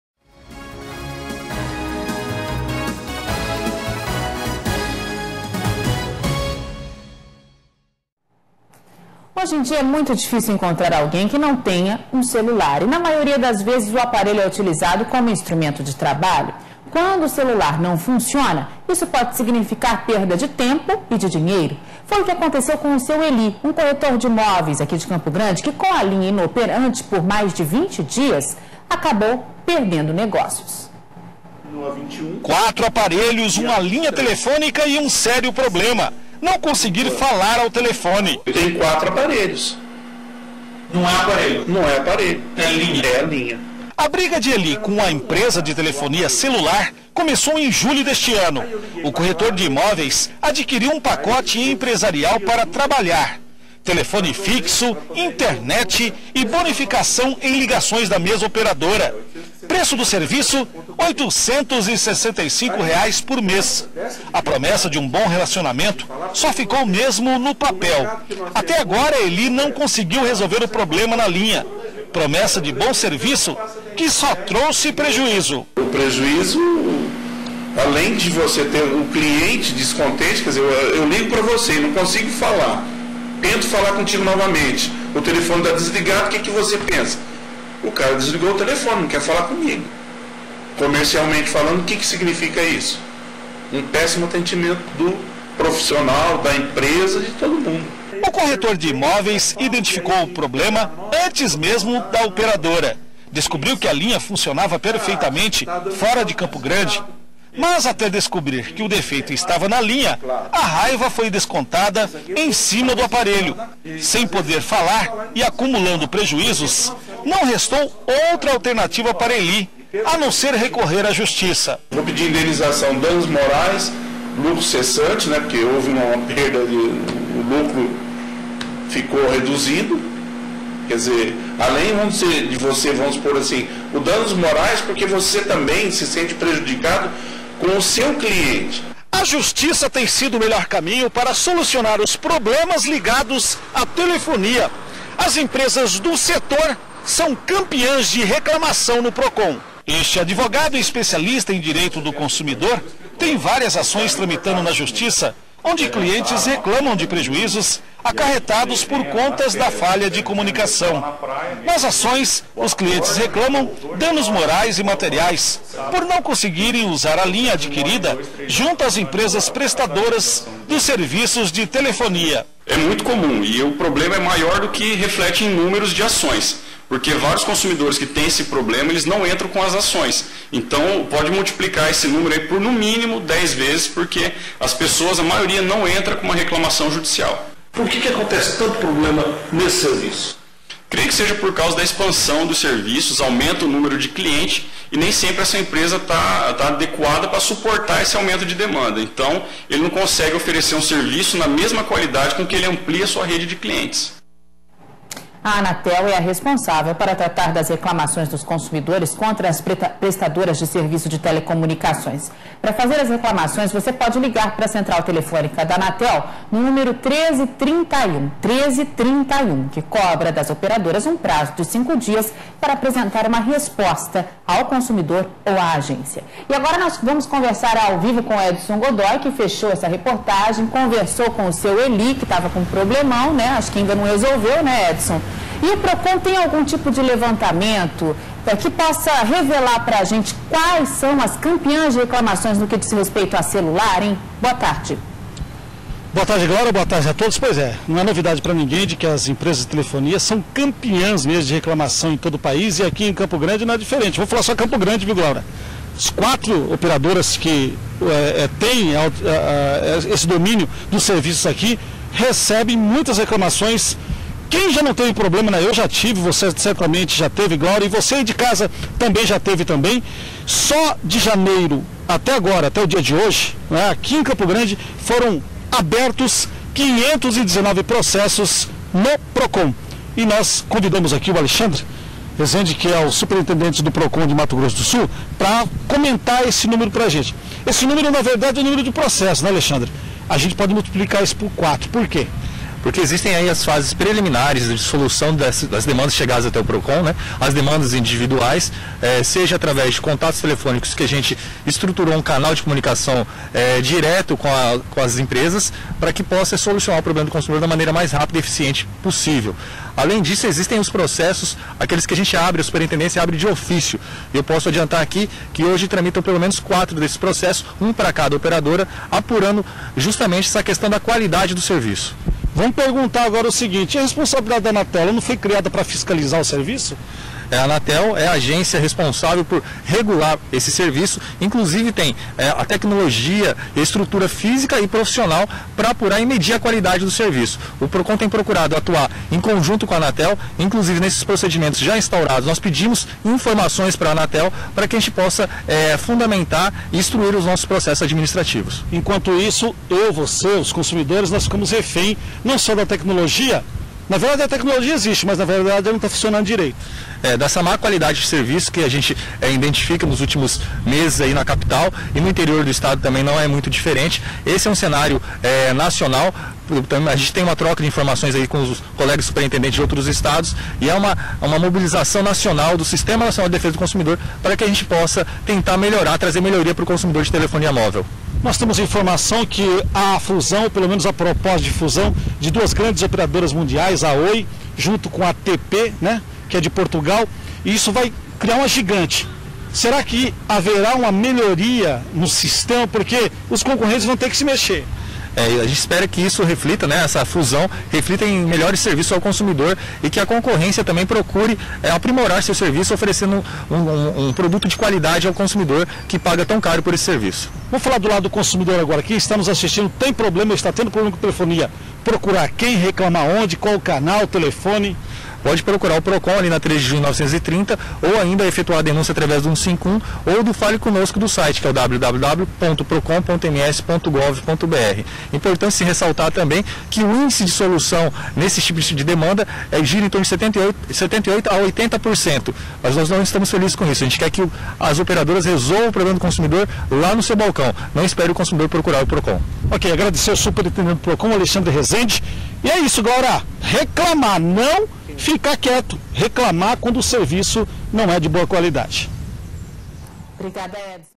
MS Record - Justiça tem sido melhor caminho para solucionar problemas ligados a serviço de telefonia
É muito difícil, hoje em dia, encontrar alguém que não tenha um celular. E na maioria das vezes, o aparelho é utilizado como instrumento de trabalho. Quando o celular não funciona, significa perder tempo e dinheiro. Foi o que aconteceu com um corretor de imóveis, que com a linha inoperante por mais de 20 dias, acabou perdendo negócios.